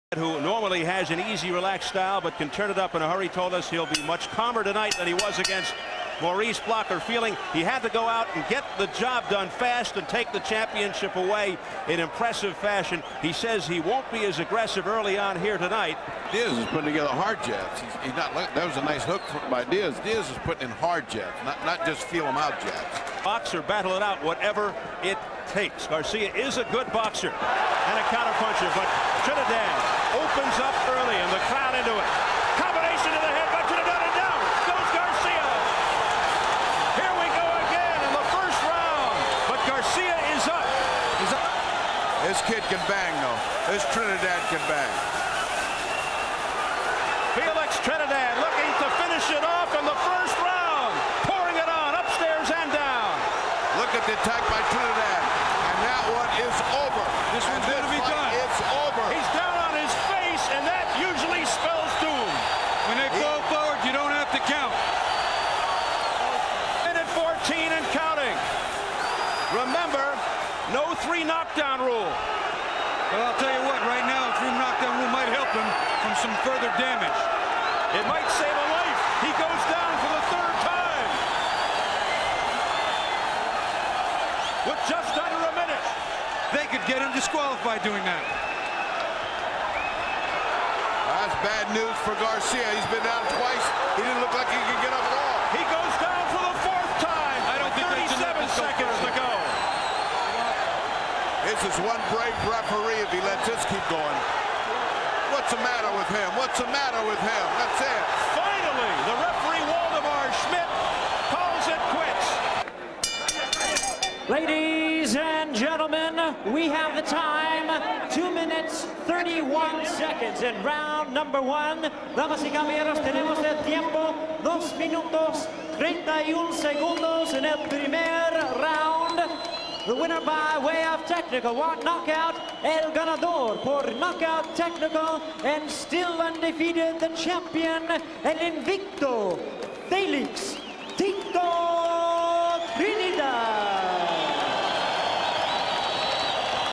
Felix Tito Trinidad Vs Luis Gabriel Garcia Highlights
Felix "Tito" Trinidad Vs Luis Gabriel Garcia. Por el Titulo Mundial Welter IBF. Trinidad hace la 1era. Defensa de su Titulo.